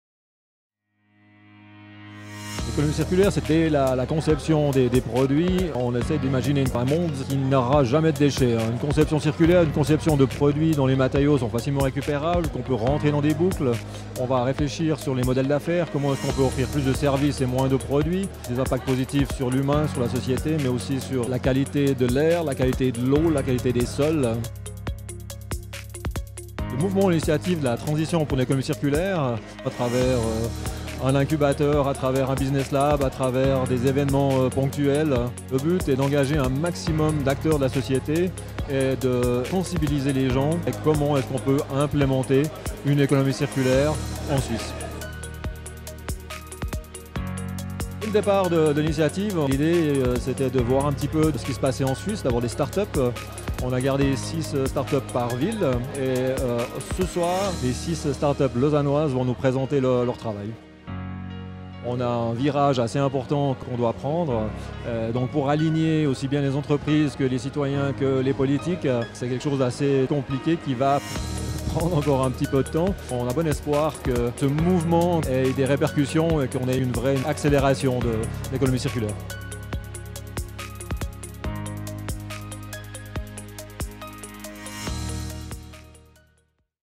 Circular Economy Transition à Lausanne - Visioniouze
Qu'est-ce que l'économie circulaire ? Réponse avec cette video tournée à Impact Hub Lausanne. 6 start-ups y présentaient leur projet dans le cadre de CE Transition - Circular Economy Transition.

Avec Laurent Maeder, spécialiste en économie circulaire

Bibou'tic, Distri'pots, Jette-pas !, RePlastic, Terrabiom, Trobak

Images: Laurent Bersier
Postproduction: Valentin Proust